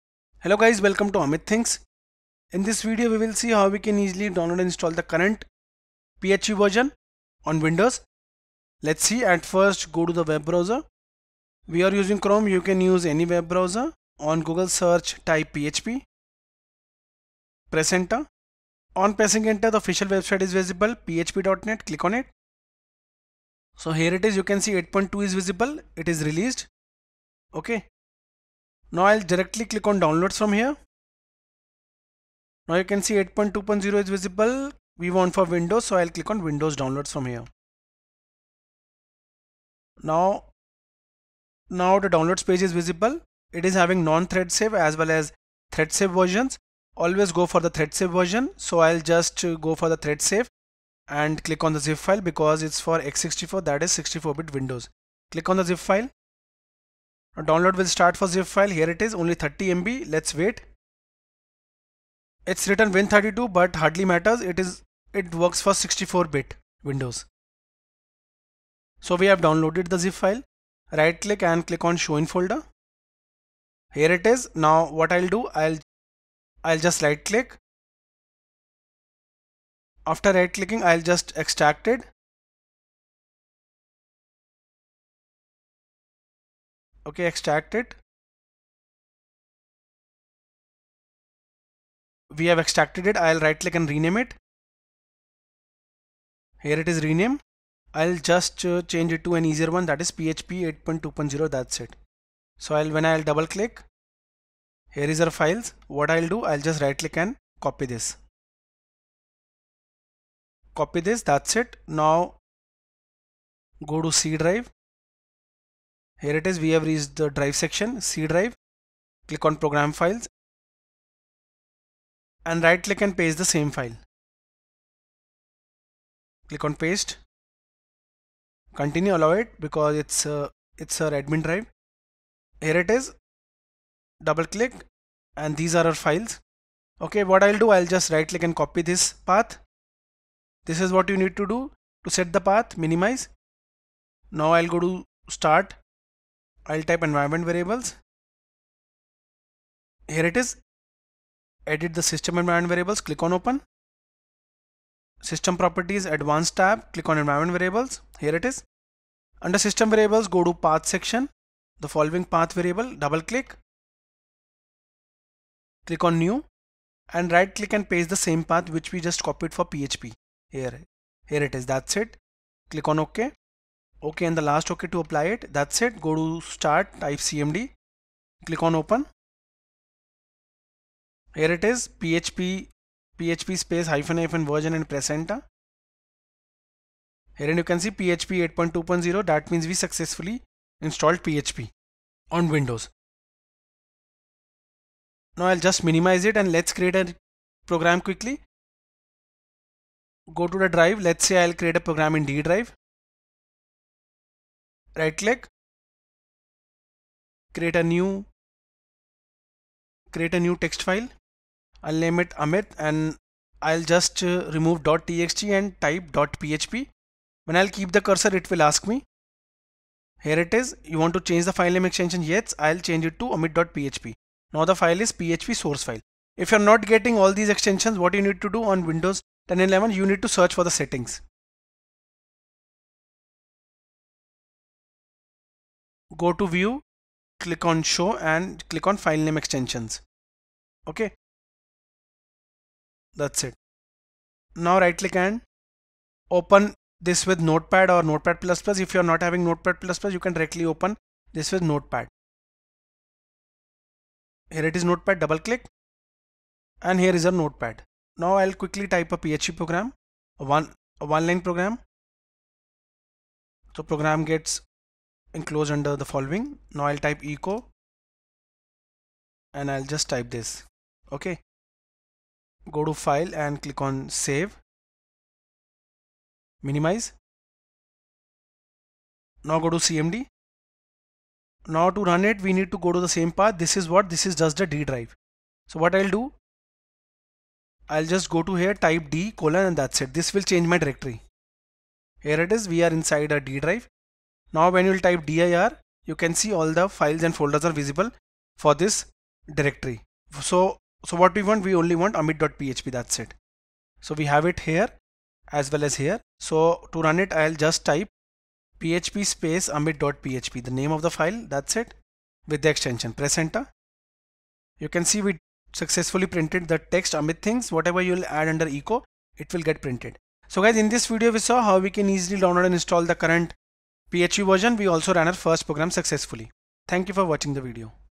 How to install PHP 8.2.0 on Windows 10 | 64 bit [2023 Updated]
Learn how to install PHP 8.2.0 on 64-bit Windows 10. We will also set the path i.e. Environment Variable. At last, we will also verify the PHP installation.